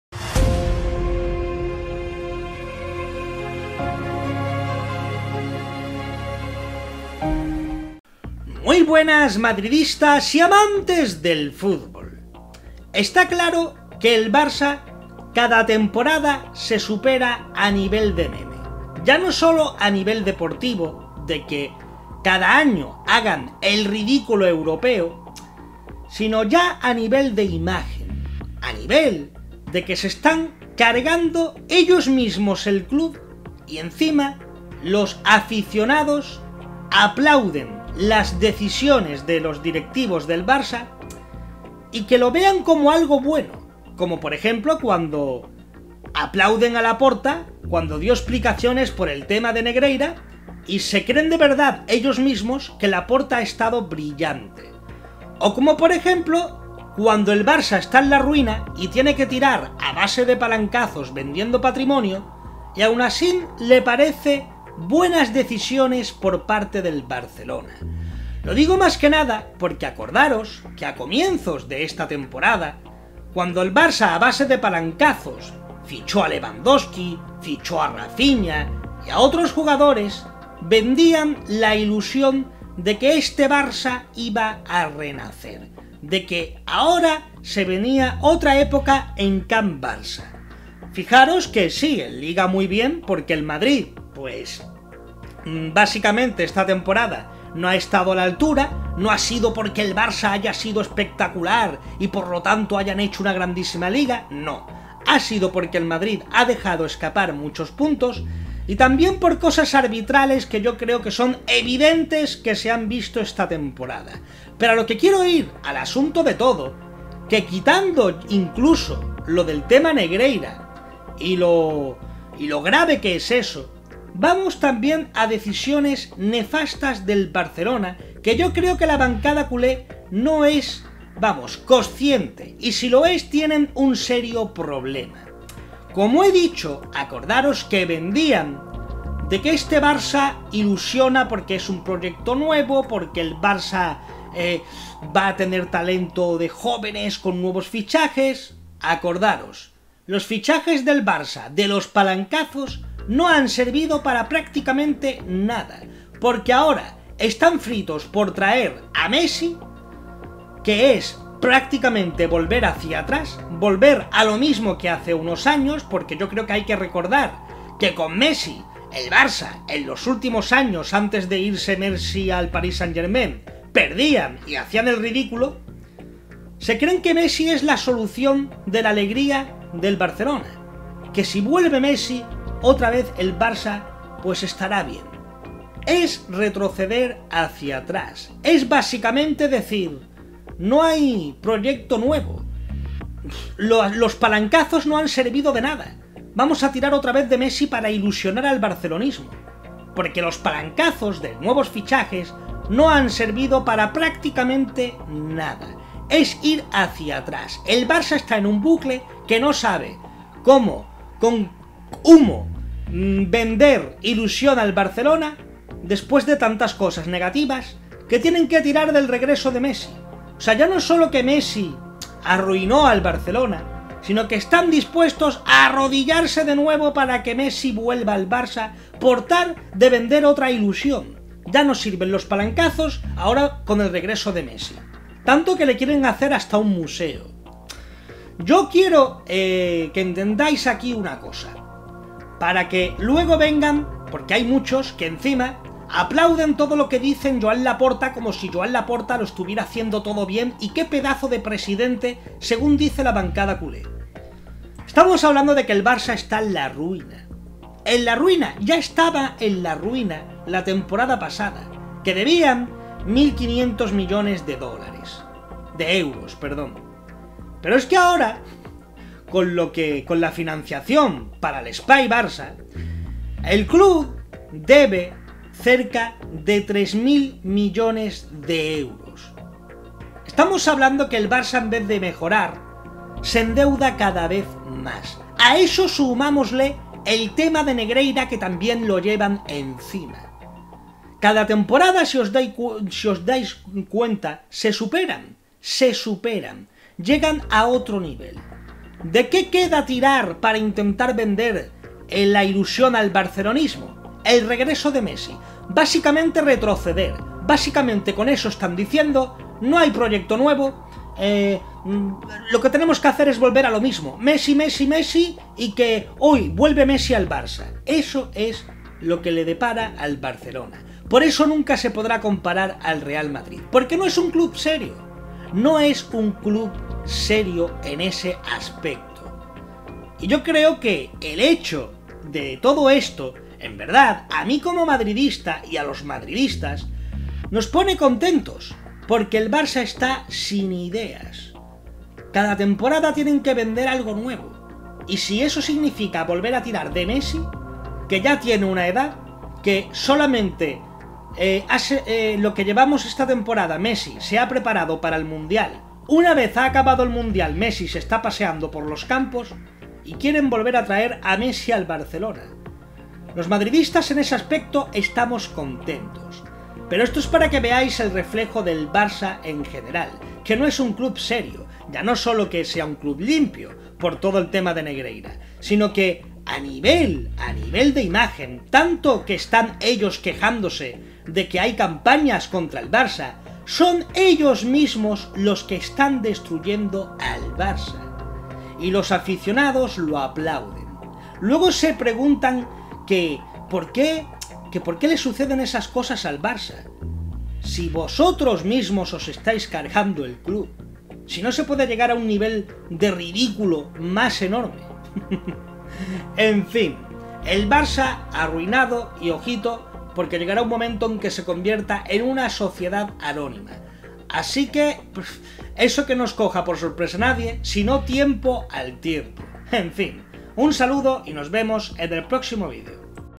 🚨 DESASTRE TOTAL! EL BARCELONA MÁS ARRUINADO QUE NUNCA Y LAS CONSECUENCIAS QUE NO QUIEREN VER ❌
En este video hago una crítica a la situación del FC Barcelona y las consecuencias que va a tener a nivel de futuro a nivel económico, las deudas, los fichajes y el tema Negreira.